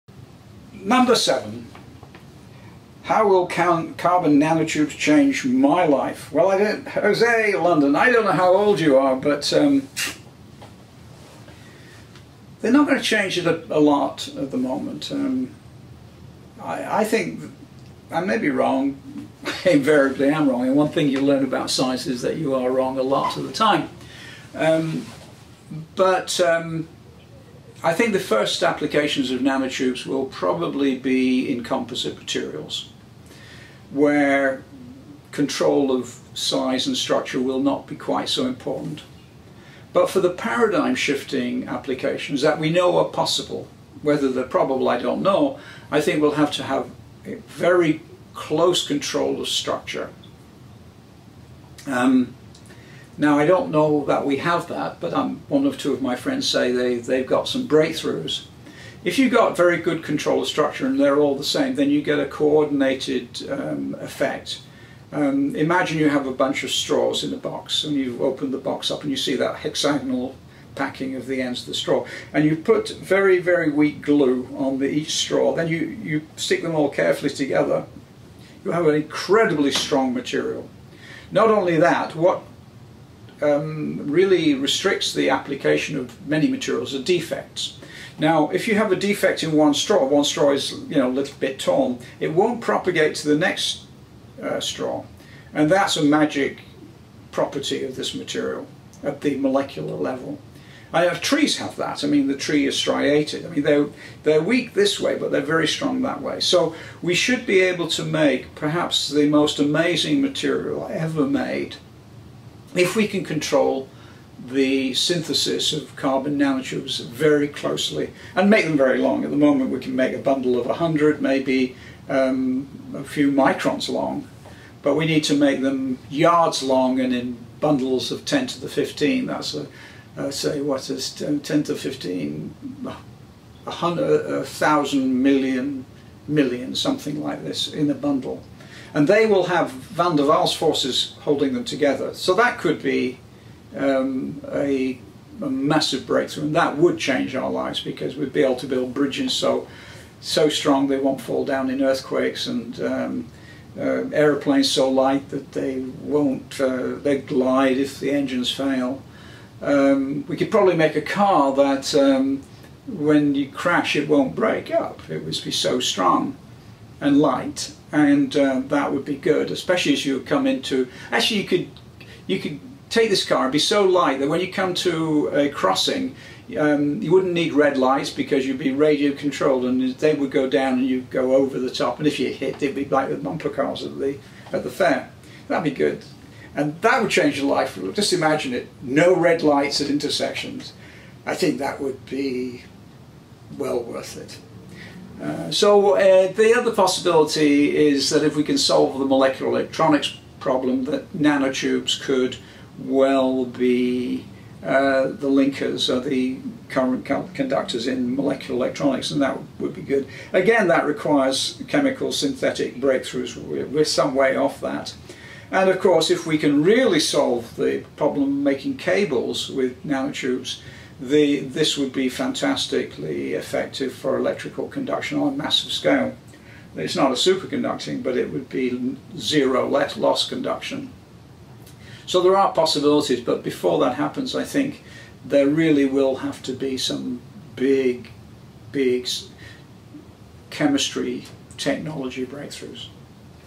Answer: How will carbon nanotubes change my life?
Sir Harry Kroto, Nobel Laureate in Chemistry 1996, has answered a selection of your video and text questions from YouTube, Facebook and Twitter. This answer is in response to a video question from José in London